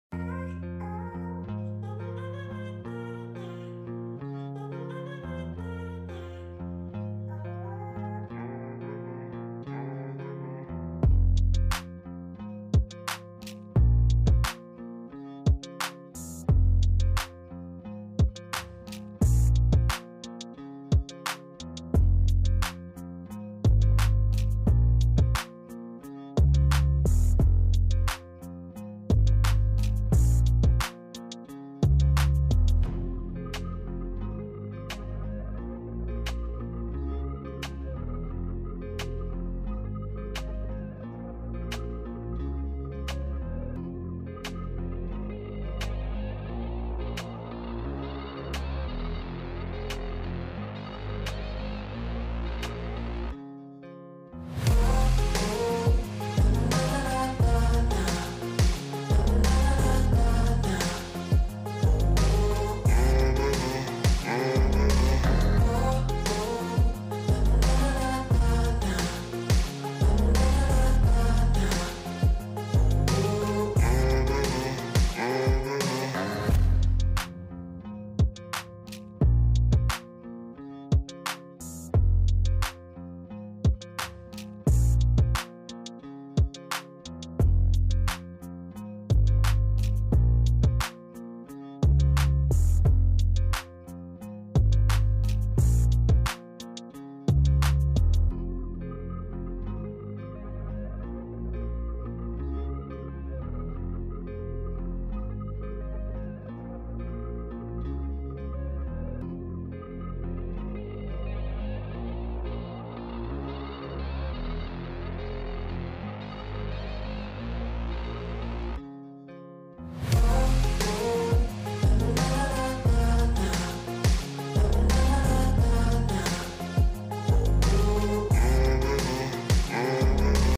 BrandNew full furnished flat sale in miyapur // Hmda Ready to move in Hyderabad
2bhk flat
Hmda appoverd
west & East face
west flat 55 Lakhs
east face 59 Lakhs
uds 38 sq yds
3 flats available
available floor 1 floor east
3rd & 4th floor west face
total flats 8 out of 5 flats sold out